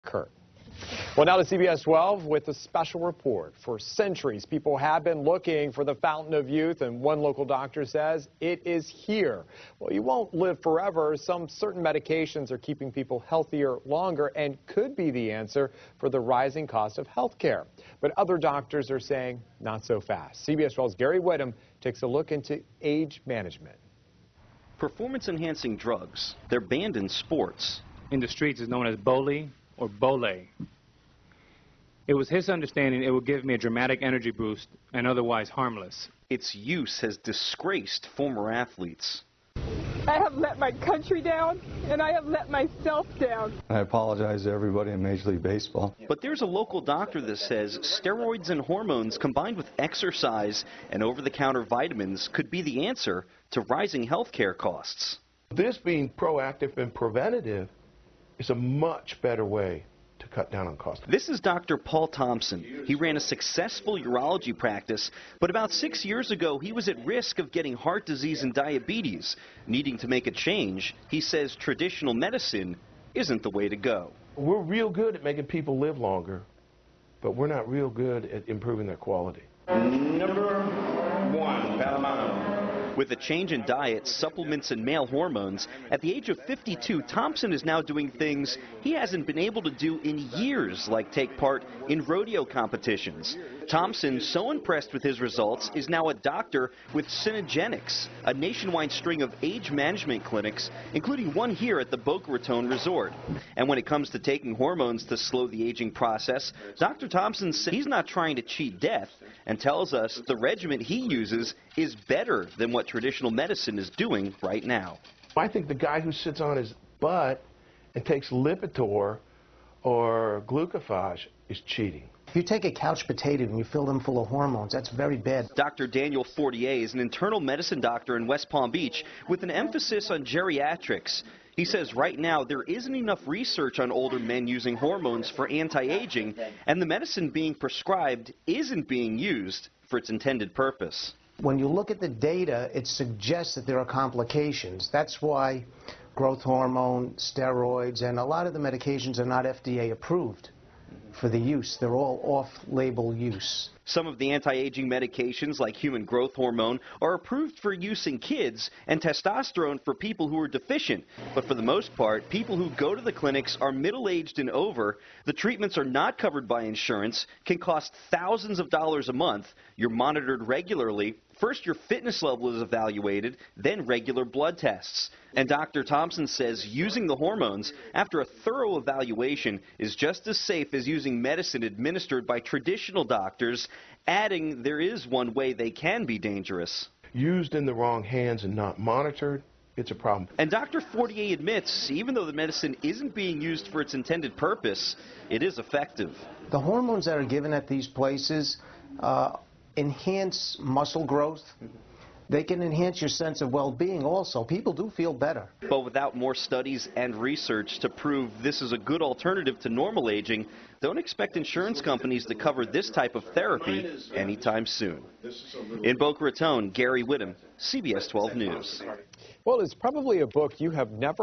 The Benefits and Risks of Male Hormone Replacement Therapy
Dr. Daniel Fortier, of Good Samaritan Medical Center, being interviewed on the benefits and risks of male hormone therapy replacement.